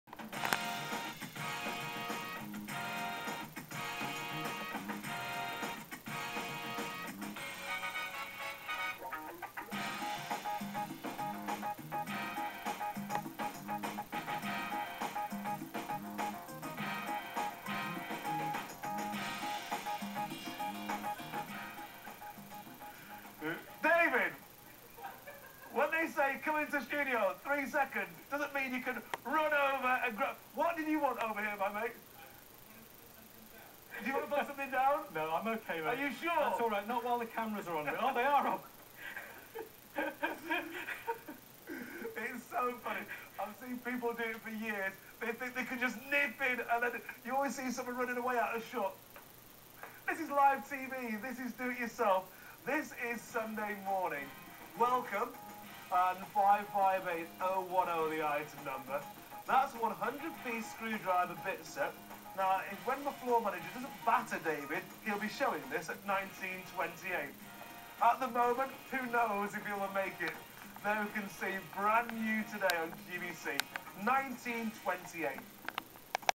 QVC UK blooper - Julian Ballantyne
Funny blooper where Julian tries to shoo the guest DIY presenter out of shot but then the opening credits end. The expression on his face is worth watching!